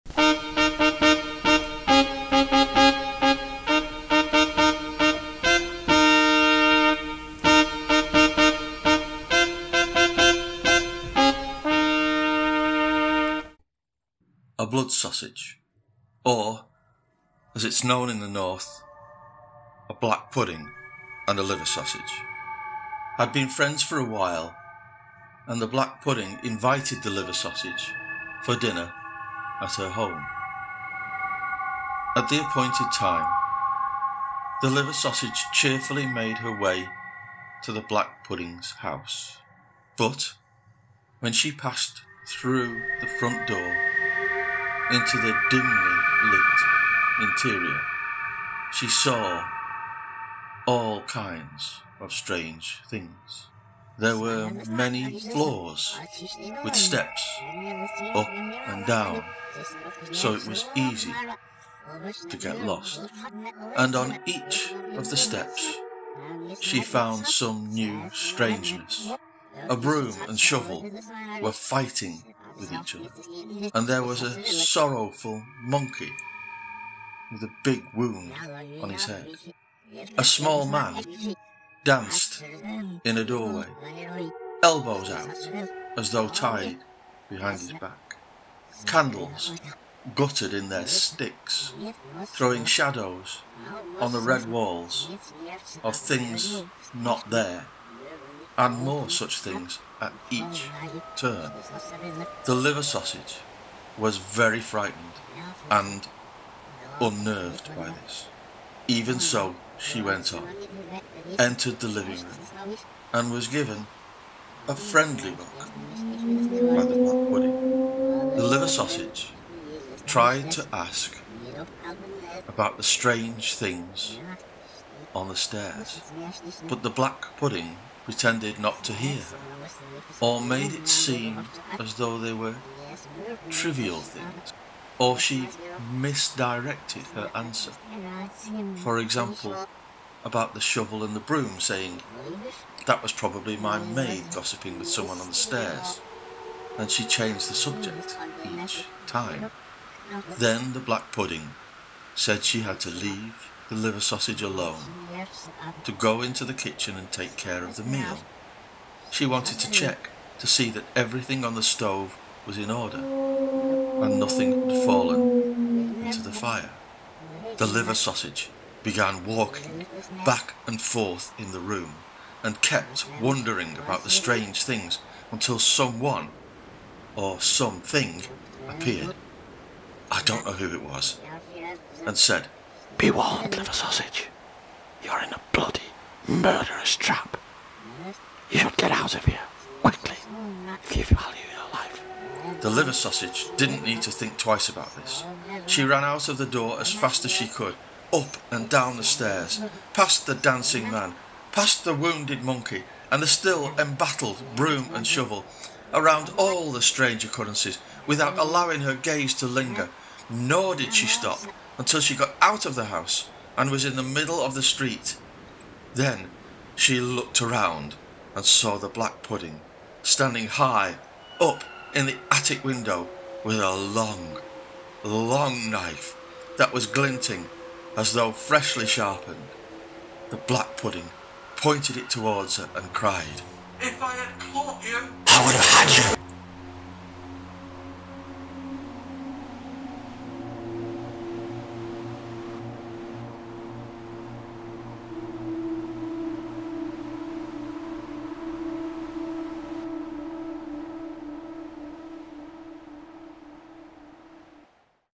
The Strange Feast
A liver sausage falls foul of a so-called friend. Spooky - probably not for the youngest.